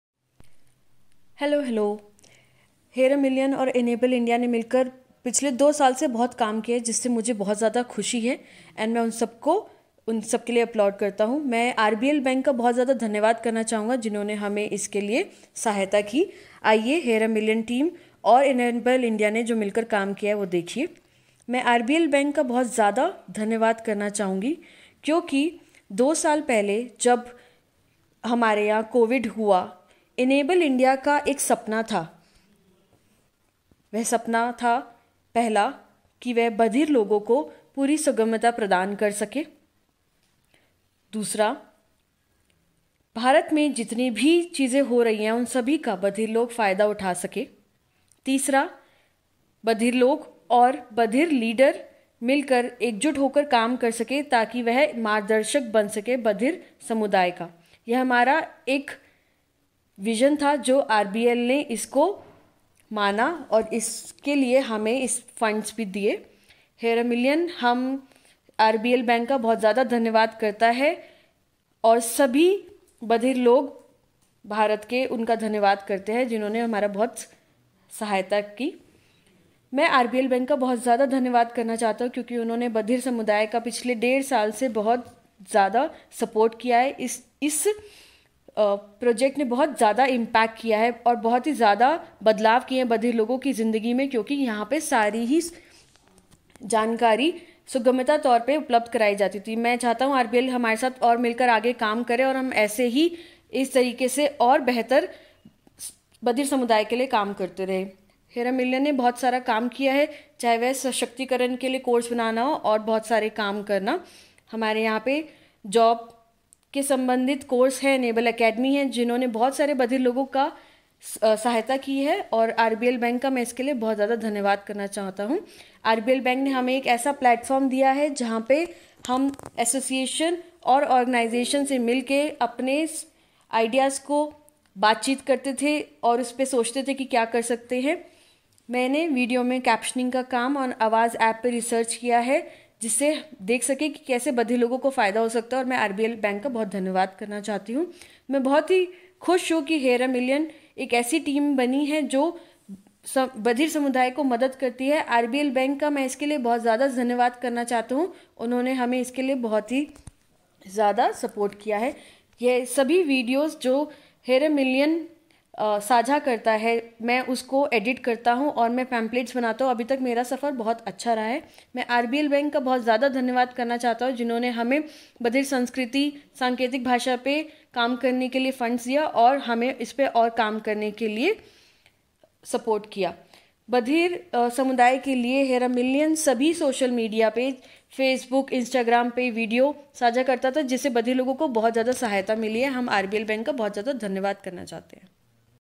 HAM and EnAble India thank RBL Bank.
All the access and awareness on social media for the Deaf Community by HAM and EI has been possible because of the financial support that RBL Bank provided. Our heartfelt gratitude to RBL Bank for all its support. We hope that the bank will continue supporting us in our endeavors of spreading access and awareness to and about the deaf community in India.